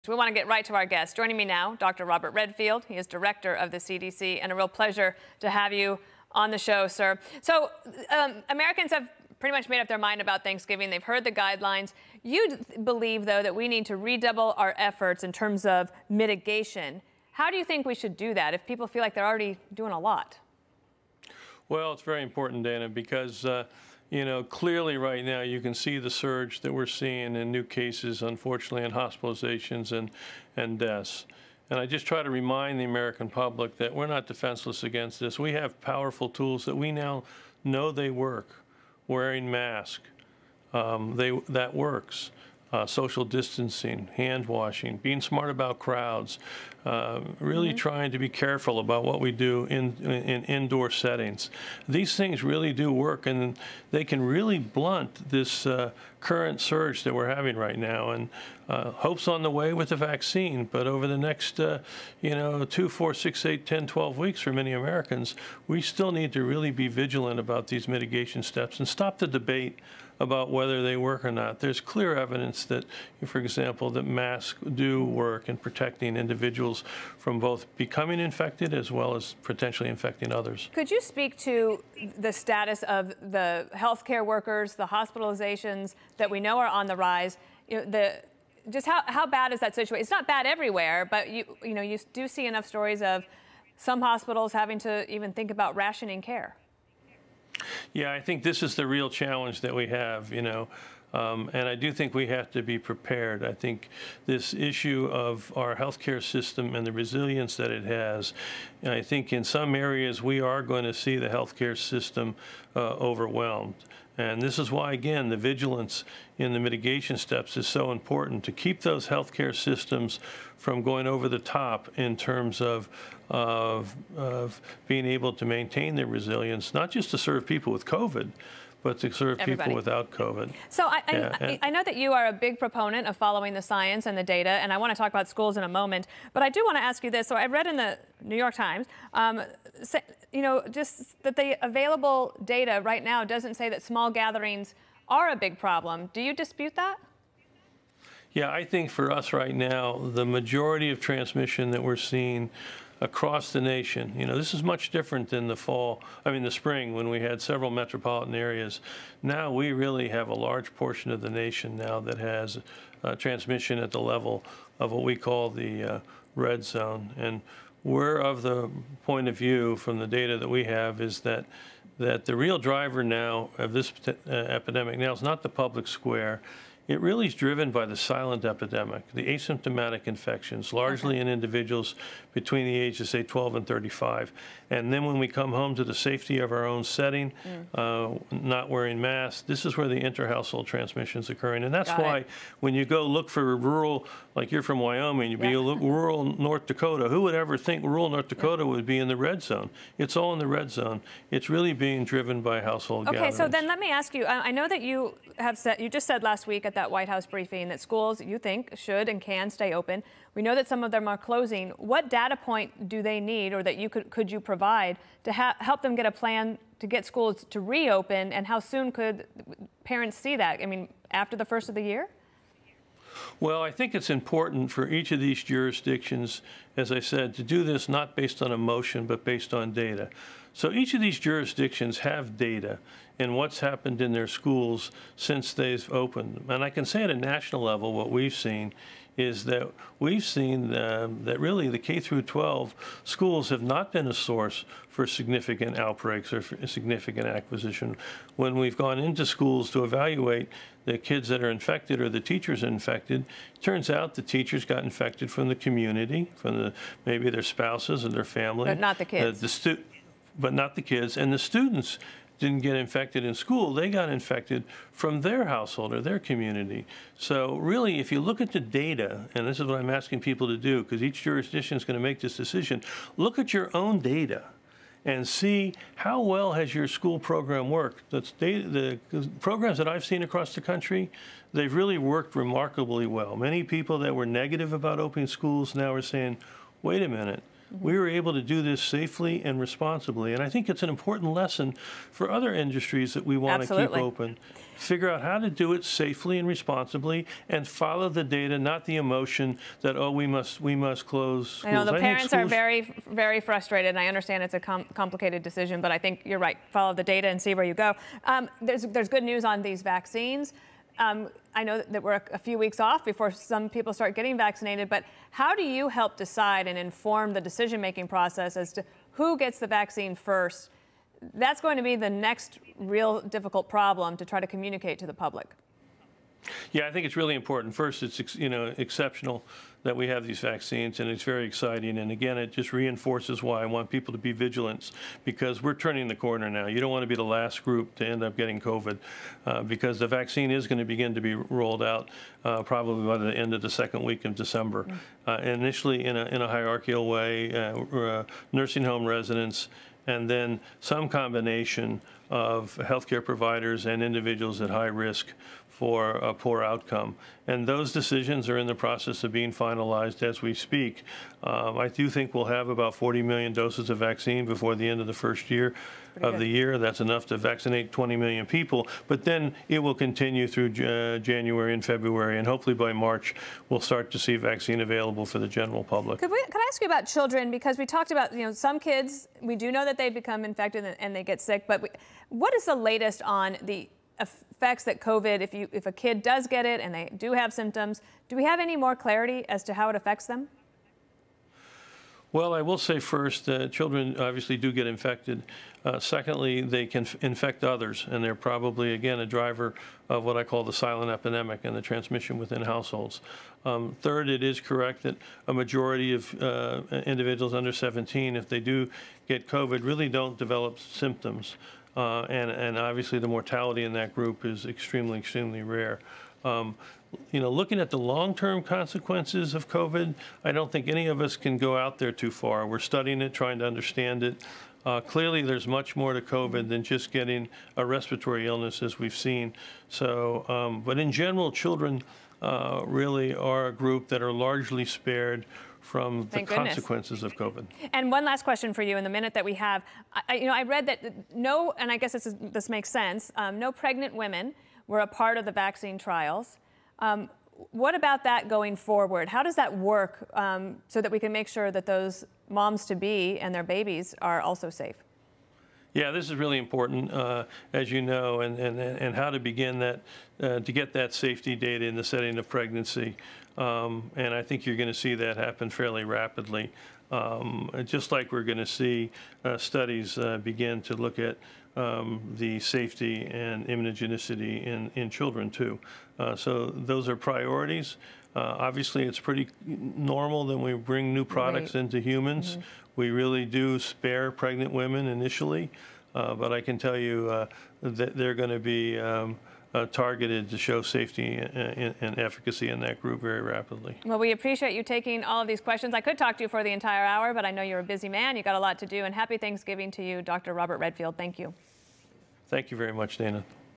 CDC Director says a coronavirus vaccine could be rolled out by December
CDC Director Robert Redfield discusses holiday travel as coronavirus cases surge and how soon the public will see a coronavirus vaccine.